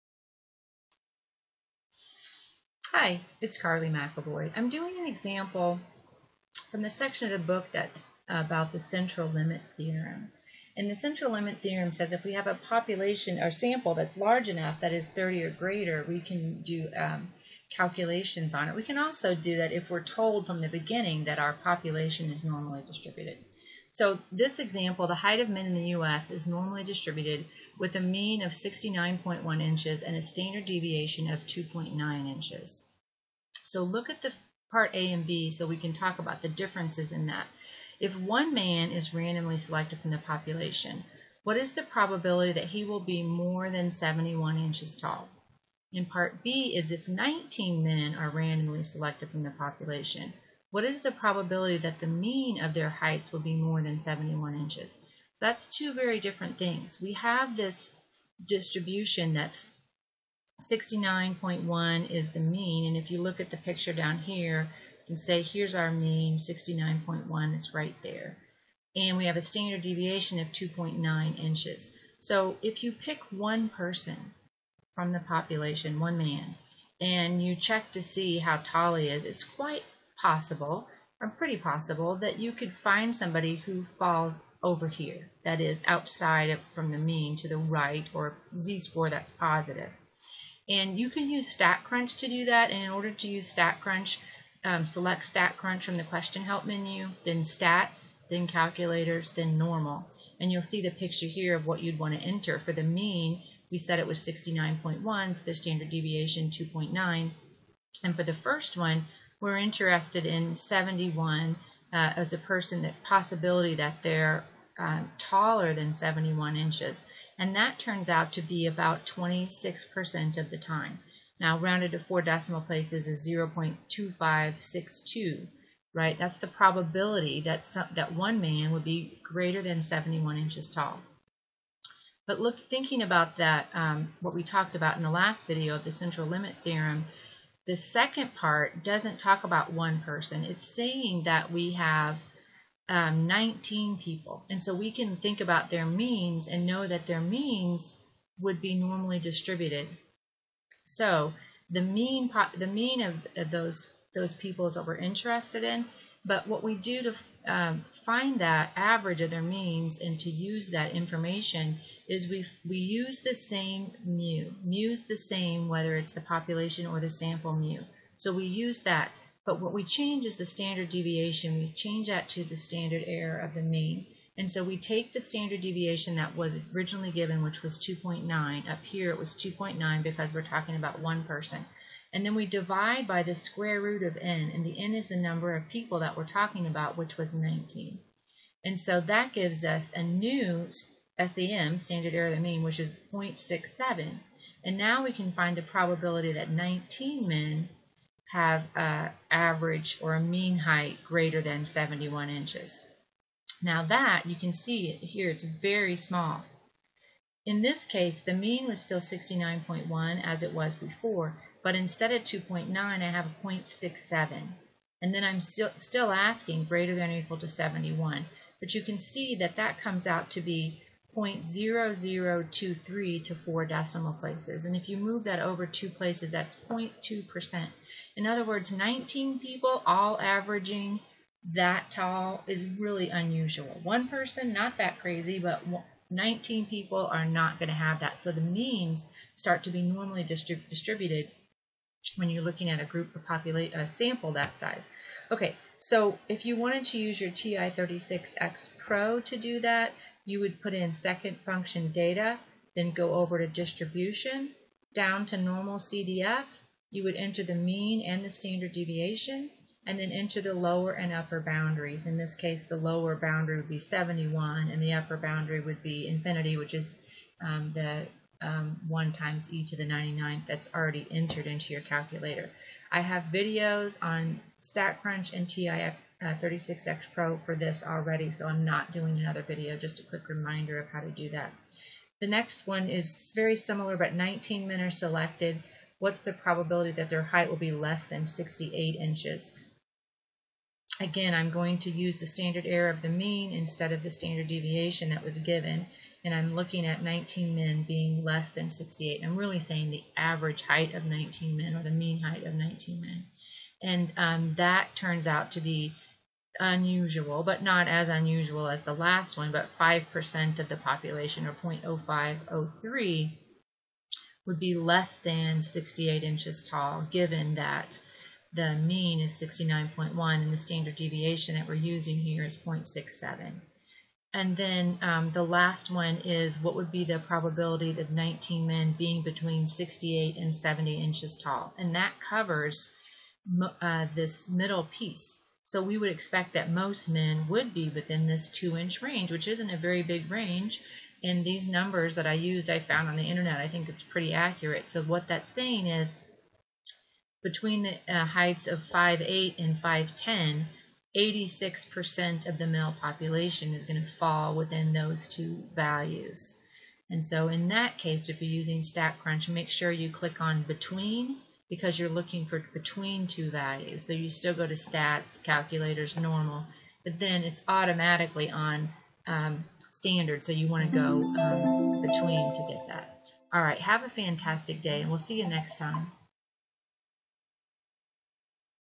Statistics: Example Standard Error of the Mean
Solve probability problems using the standard error of the mean from the Central Limit Theorem.